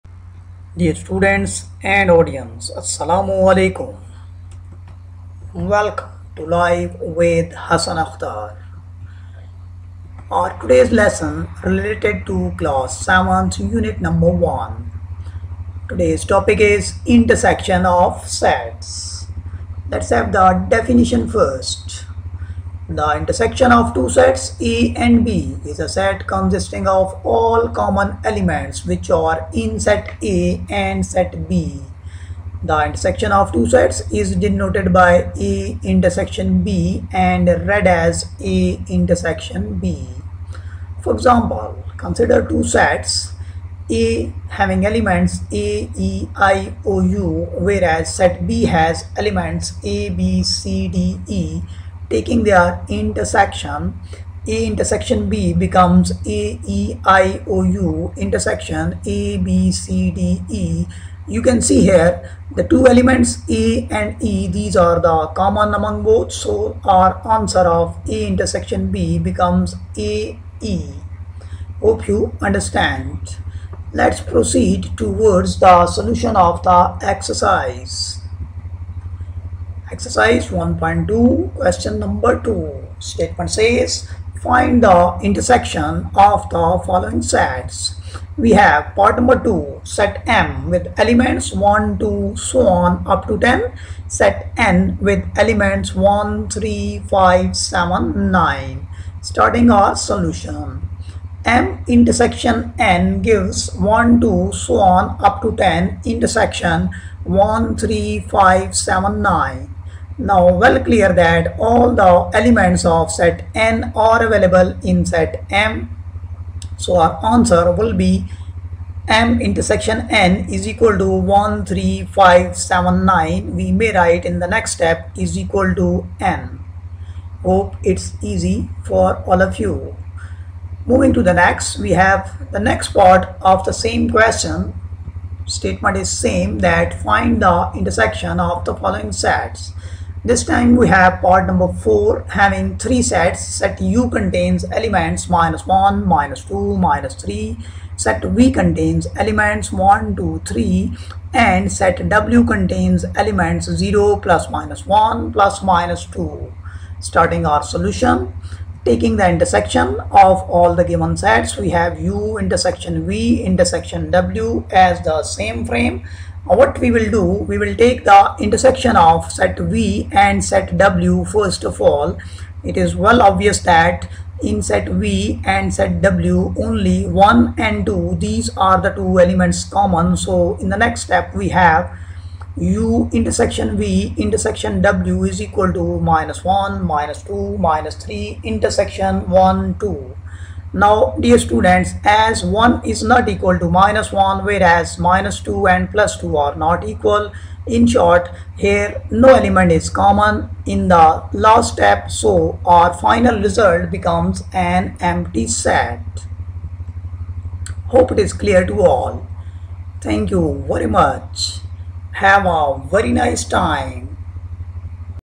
Intersection of Sets || Class 7th Exercise 1.2 Question 2 || By Sir Hassan Akhtar
Definition: Intersection of Sets
Exercise 1.2
Solution of Question 2.
Writing solutions of questions in
Tabular form of sets.
Writing common elements of given sets.